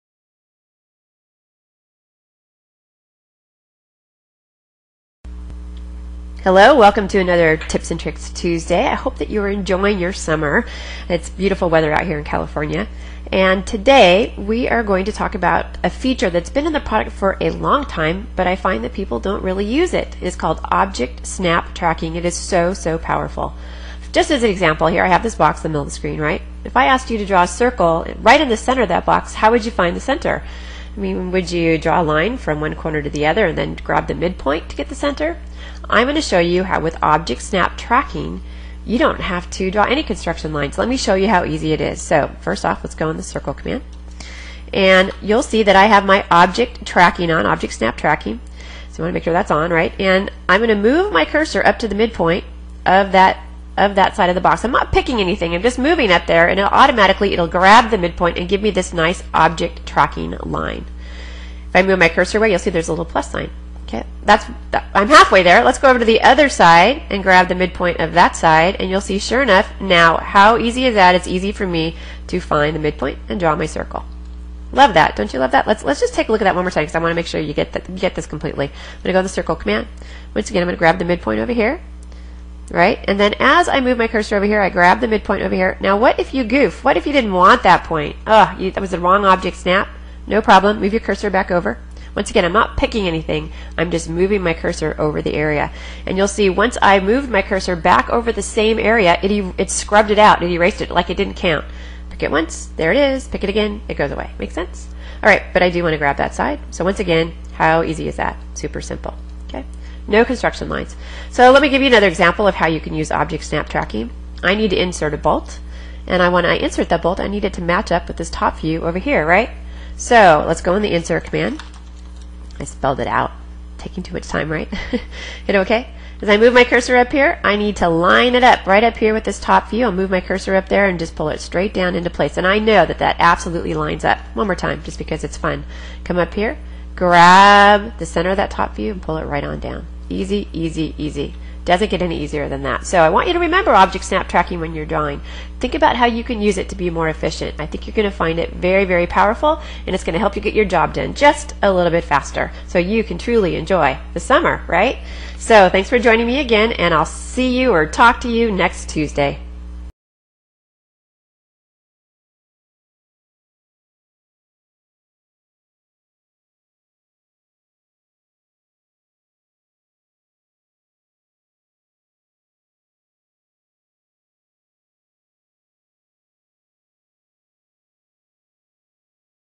AutoCAD Tip -- Object Snap Tracking (Lynn Allen/Cadalyst Magazine)
Use the Layiso command in recent versions of AutoCAD to hide or lock all layers except those of the selected objects. Layuniso brings back the isolated layers.